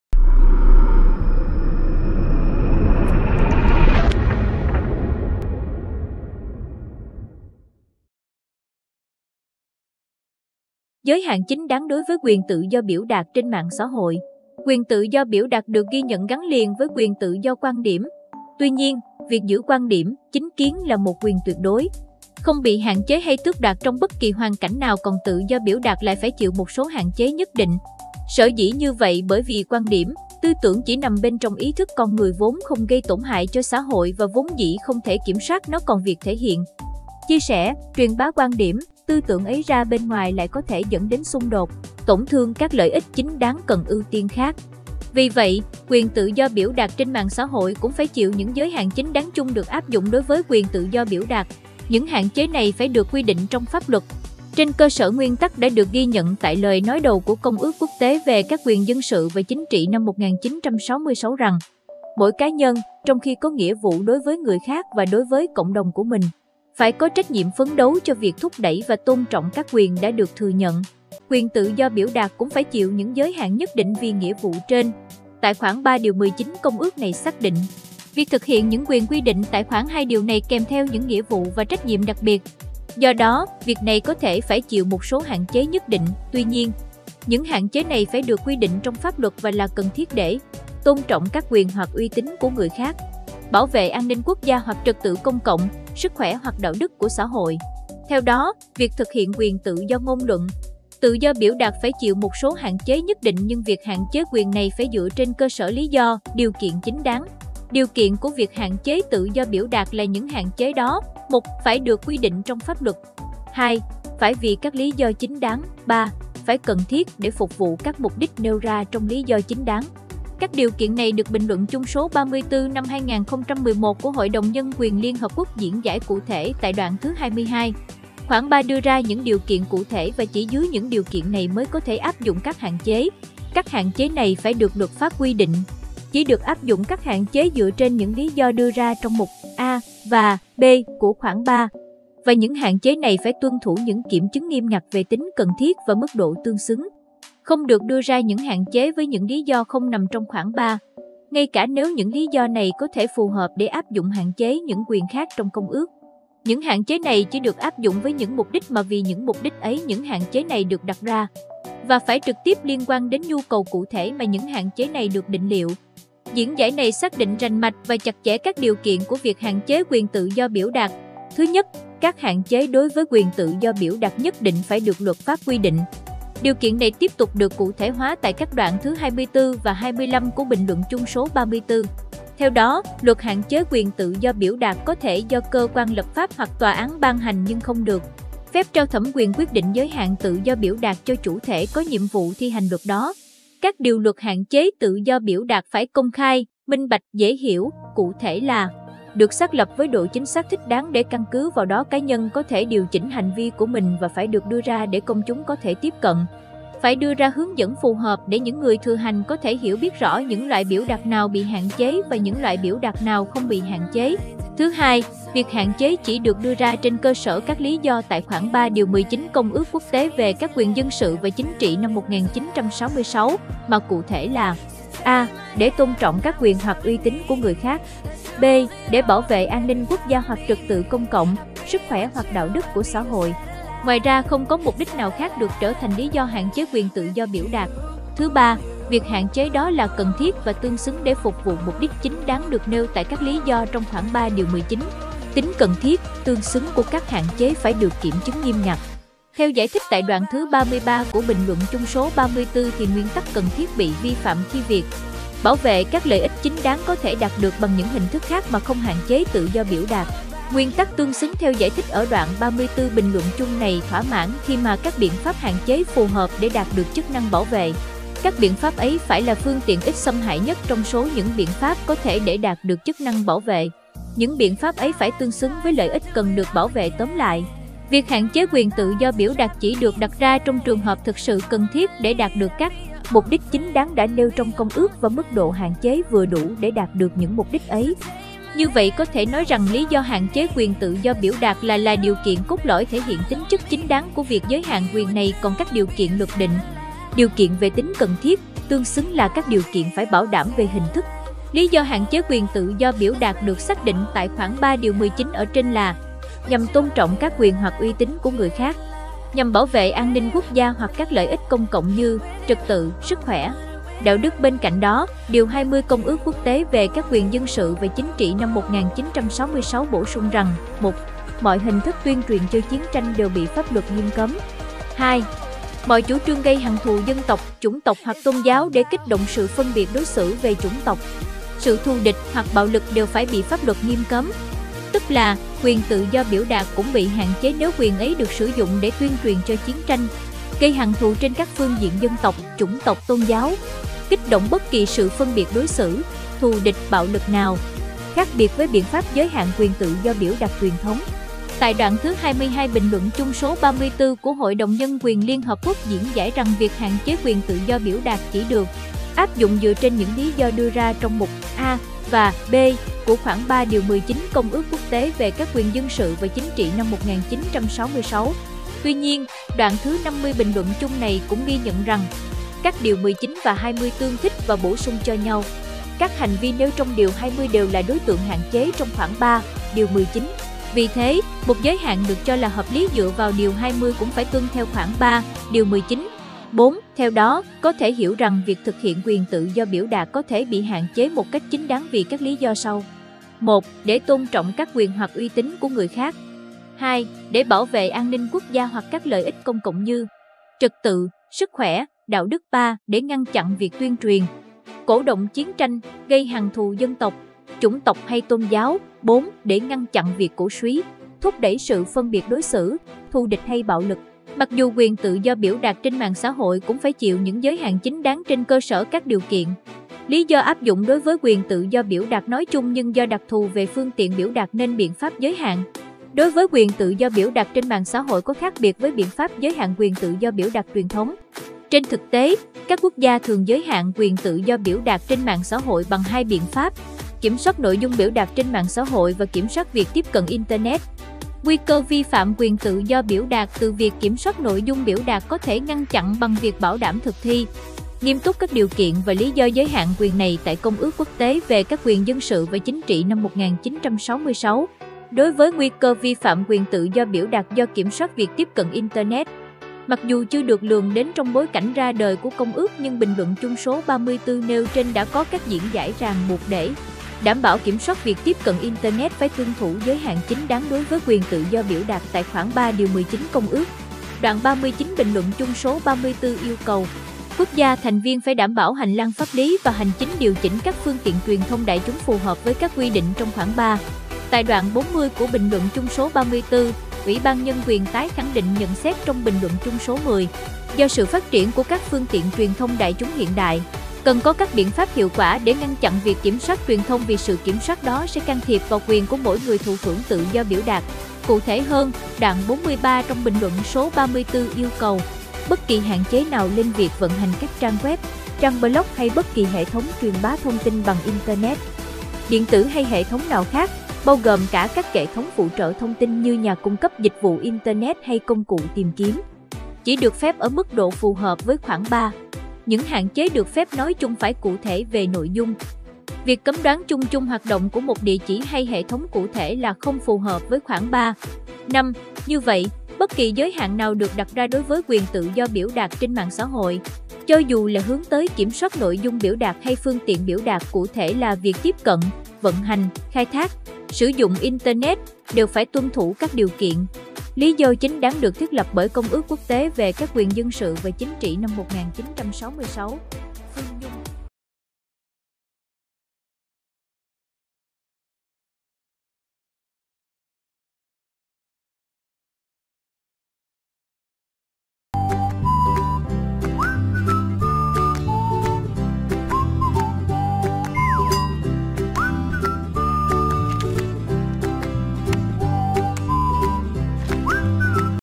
Giới hạn chính đáng đối với quyền tự do biểu đạt trên mạng xã hội
Quyền tự do biểu đạt được ghi nhận gắn liền với quyền tự do quan điểm, tuy nhiên, việc giữ quan điểm, chính kiến là một quyền tuyệt đối, không bị hạn chế hay tước đoạt trong bất kỳ hoàn cảnh nào còn tự do biểu đạt lại phải chịu một số hạn chế nhất định. Sở dĩ như vậy bởi vì quan điểm, tư tưởng chỉ nằm bên trong ý thức con người vốn không gây tổn hại cho xã hội và vốn dĩ không thể kiểm soát nó còn việc thể hiện, chia sẻ, truyền bá quan điểm, tư tưởng ấy ra bên ngoài lại có thể dẫn đến xung đột, tổn thương các lợi ích chính đáng cần ưu tiên khác. Vì vậy, quyền tự do biểu đạt trên mạng xã hội cũng phải chịu những giới hạn chính đáng chung được áp dụng đối với quyền tự do biểu đạt.